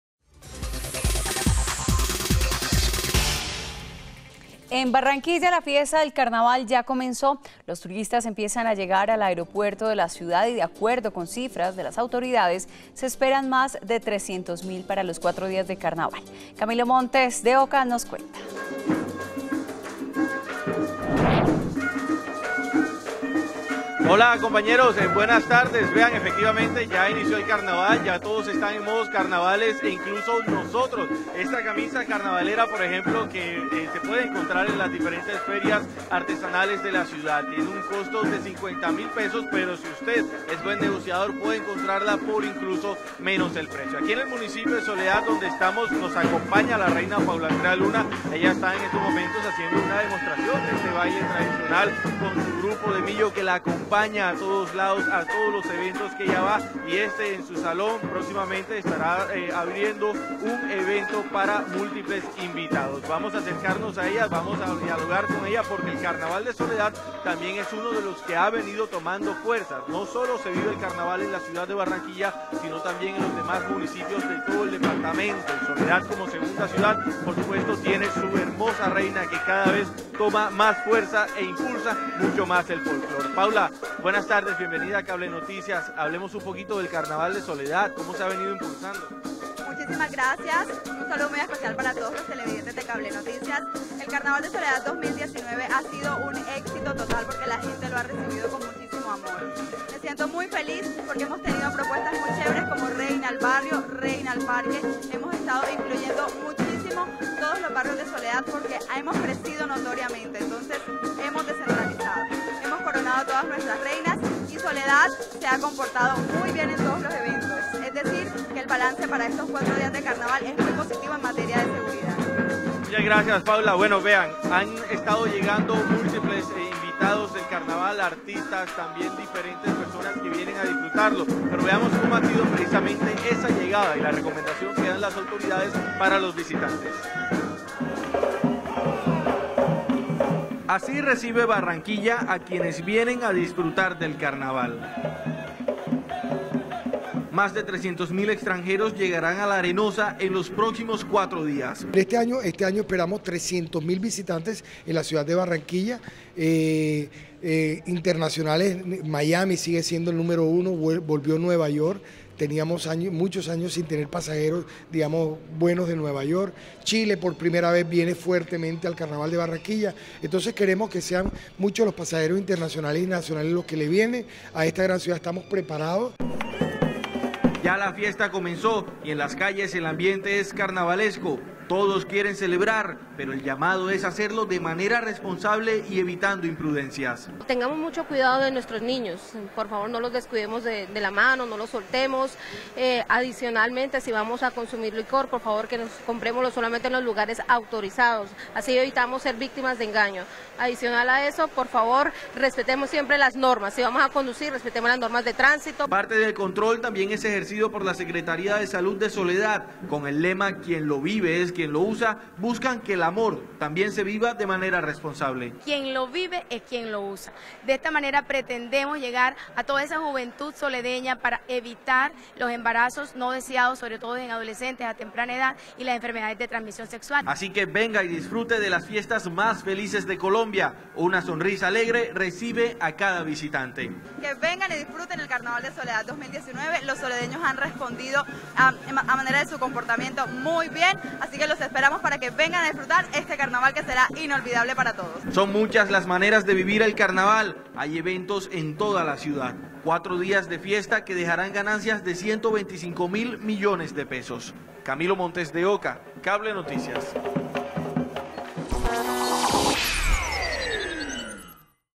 ¡Con rumba!, Así reciben a los turistas en aeropuerto de Barranquilla
Con bombos y platillos, así reciben reciben a los turistas que llegan al Aeropuerto de Barranquilla por el Carnaval de Barranquilla que inició desde anoche con la mágica coronación de la reina de estas festividades.